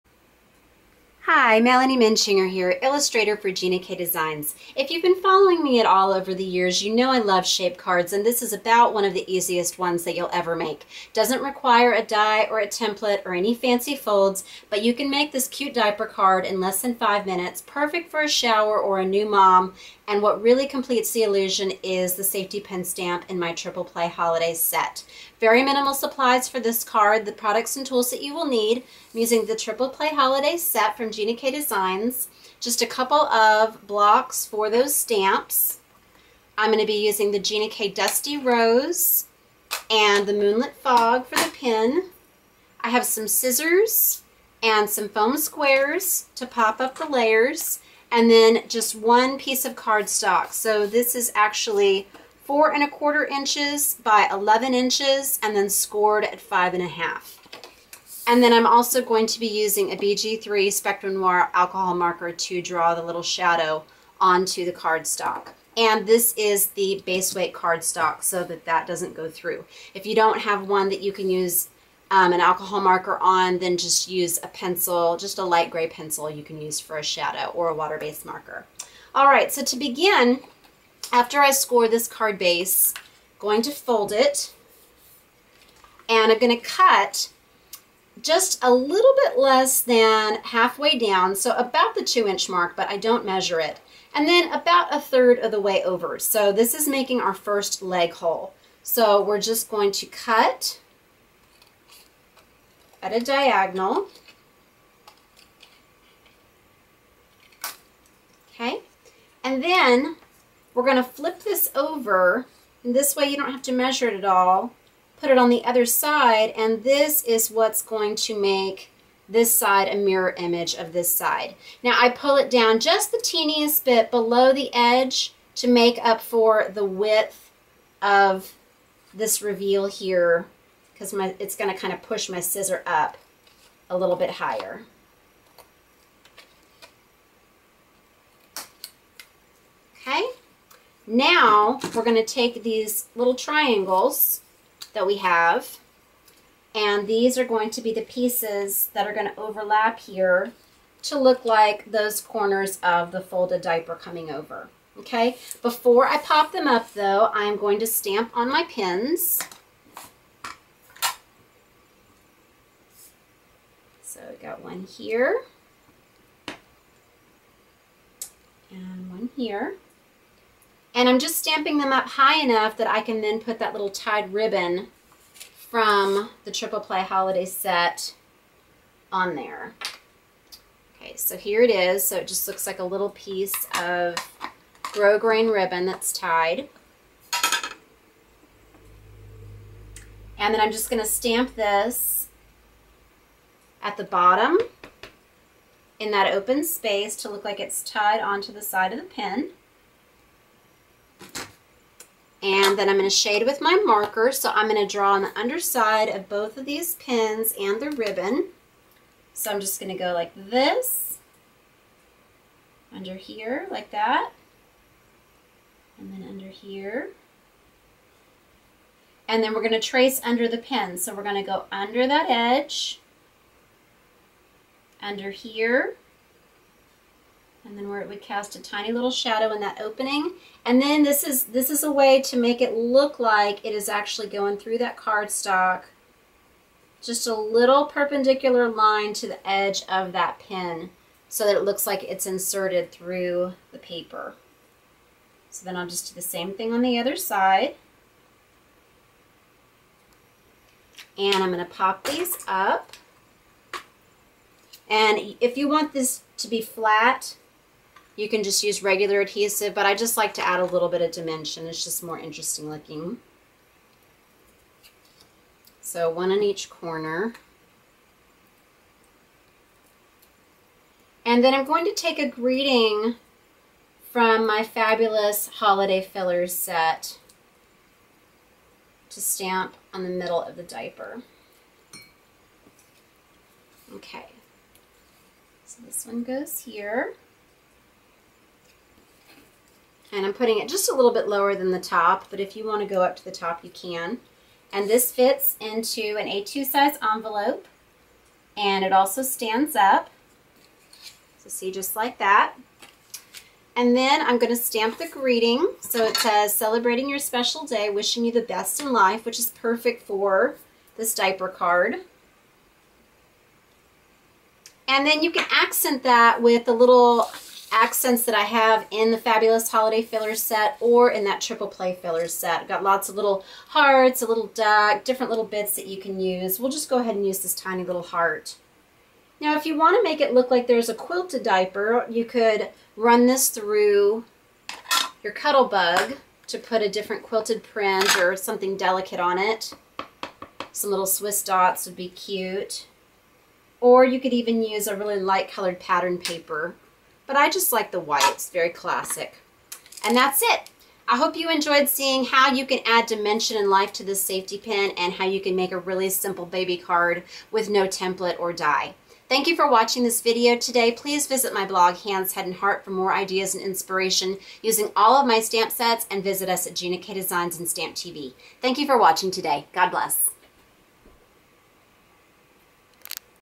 How to Make a Simple 3D Diaper Card-No Template Needed
Here’s a card you can whip up in less than 5 minutes for a new mom  or baby shower, no die or template needed! The safety pin and ribbon stamps from Triple Play Holiday are all you need to complete the illusion on this adorable 3 D card. Emboss the safety pin for added realism or dry emboss a pattern to add texture to the diaper.  You might also swap out the white for pastel colors or substitute delicate patterned papers. Please enjoy, and thank you in advance for the 'likes"!

Supplies:

Gina K. Designs Triple Play Holiday, Fabulous Holiday Fillers (greeting) stamp sets
Gina K. Pure Luxury White cardstock
Gina K. Moonlit Fog and Dusty Rose ink pads
Spectrum Noir B3 marker
Scissors, foam adhesive squares